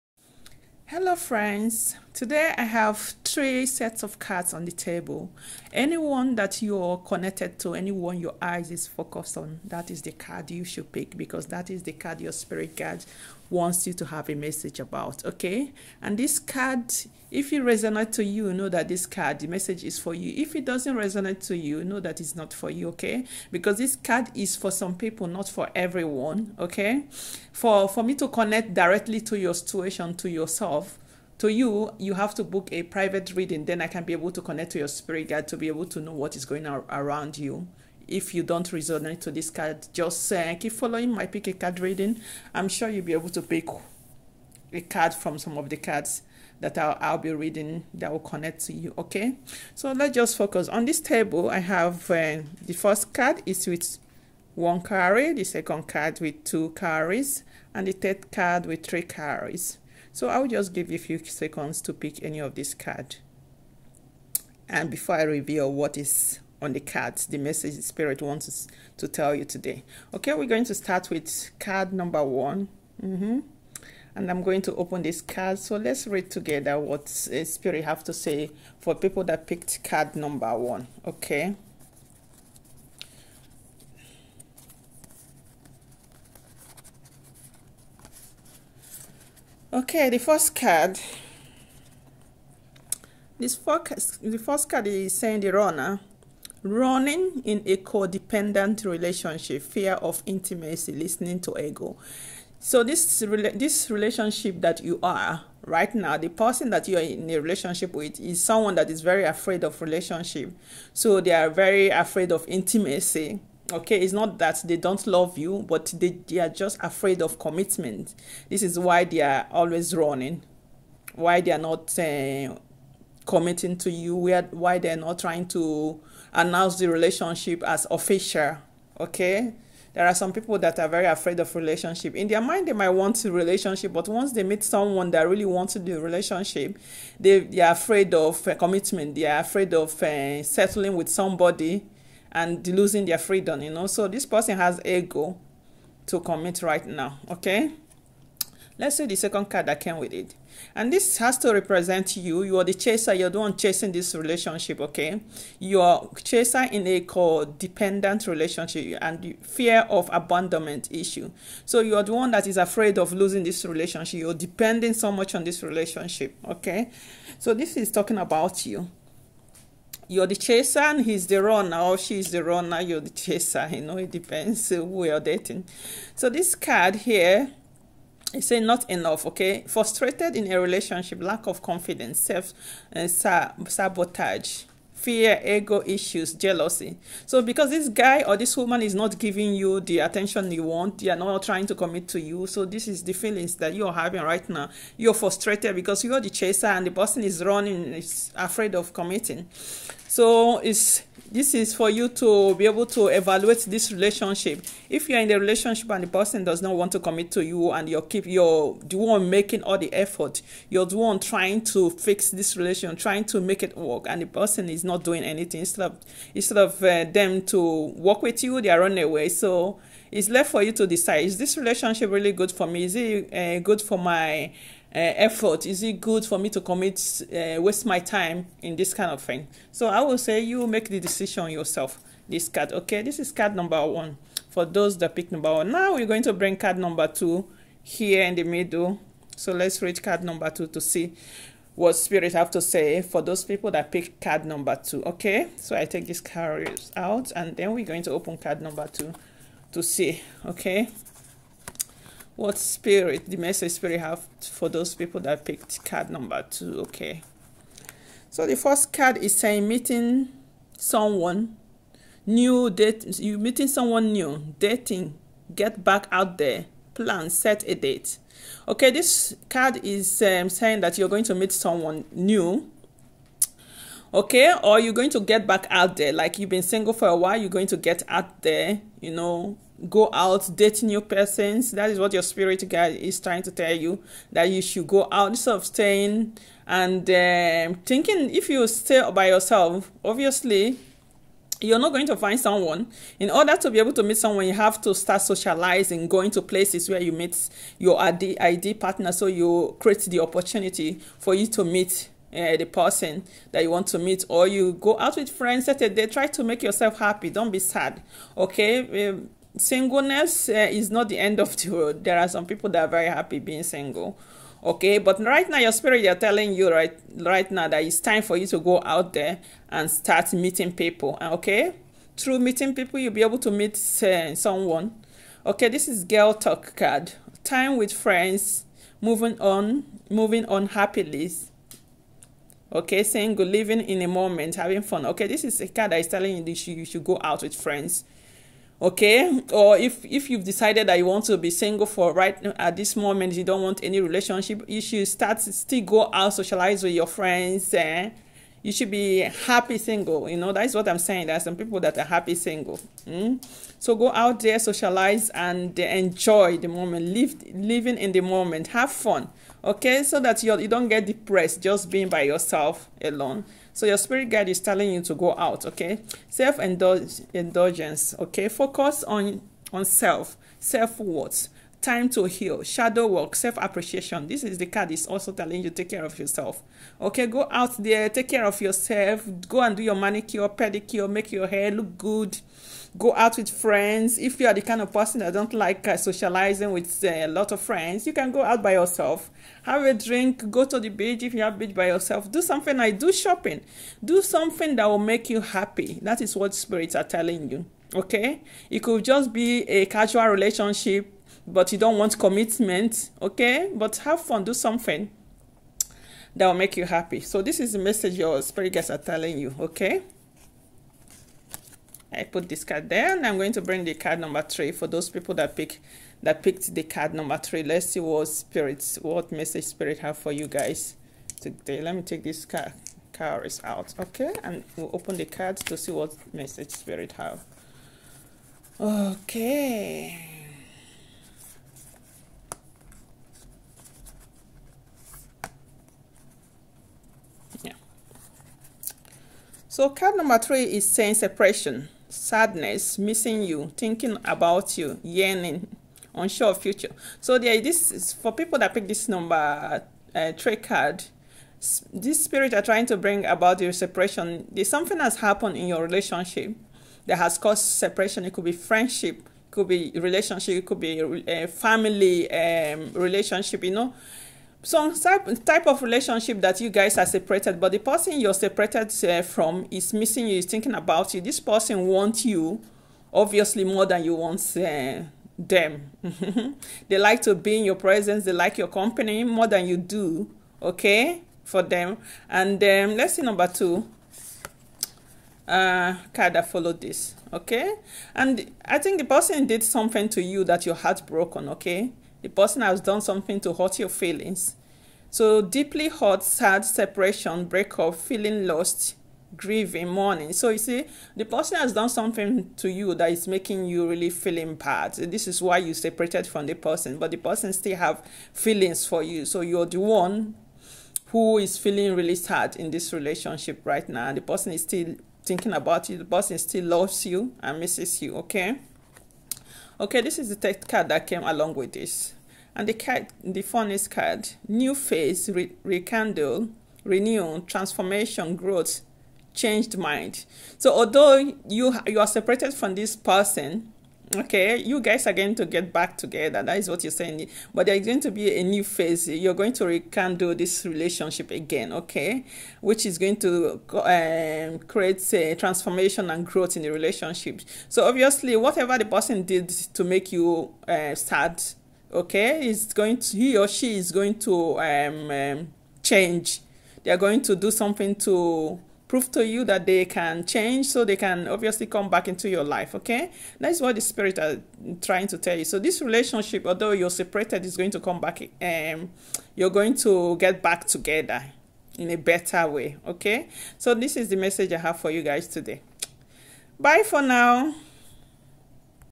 🔮 Love & Relationship Reading 🧿. ❤️Does He/She Feel The Same As You? Findout (Pick a Card)
Pick a card to see the message spirits have for you today. To book private reading , book for sessions via my